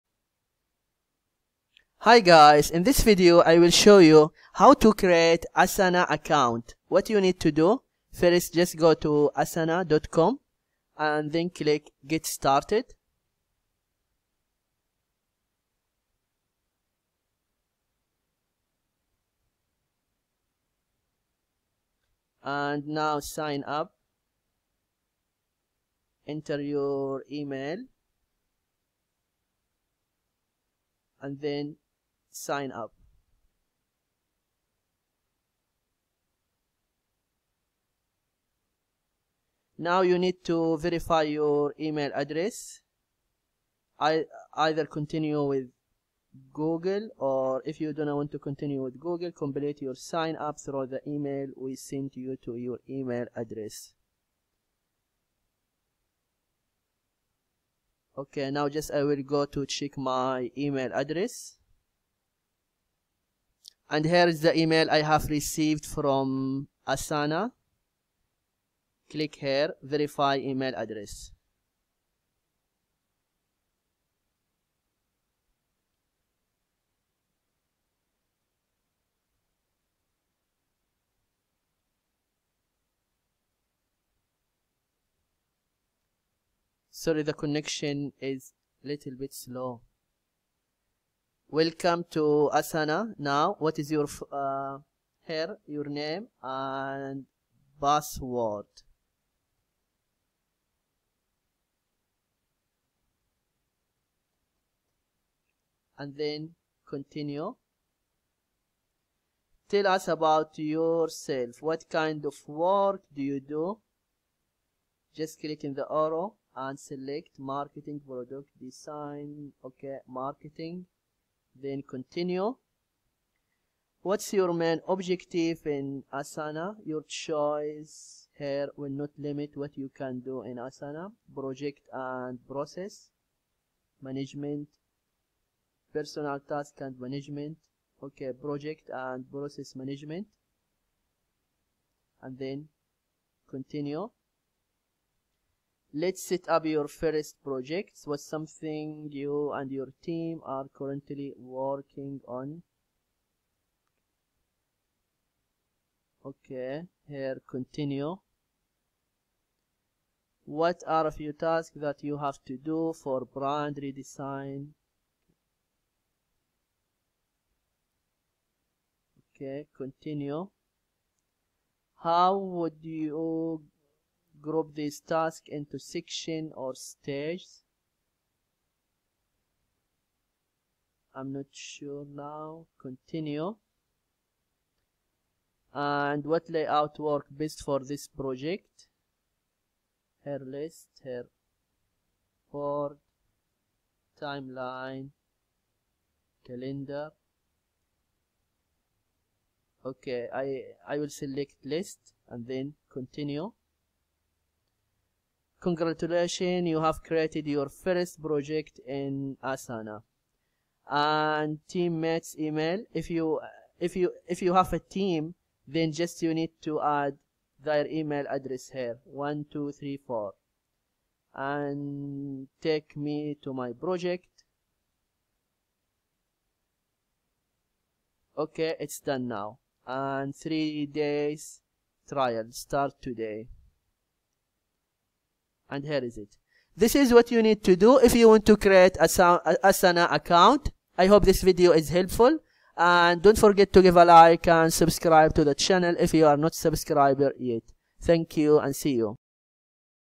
How To Create Asana Account Easy 2022
Stay secure online and hide from your ISP with IVACY VPN!

In this video, I will show you How To Create Asana Account Easy 2022